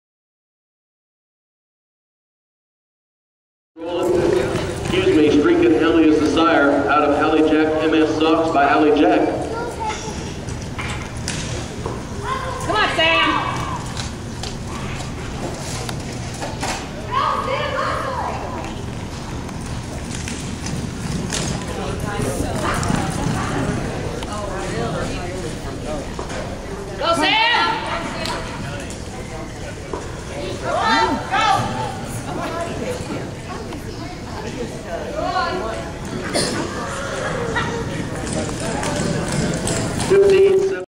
STREAKIN ALI JACK
Congratulations to Streakin Ali Jack at Cornhusker Breeders Futurity!    Owners, Justin and Lori Robinson Rider, Samantha Flannery Finishing 6th in the Average! Selling a FULL sister STREAKIN DOUBLEFROST Lot 46 at the Fulton Performance Horse Sale Aug. 22, 2014!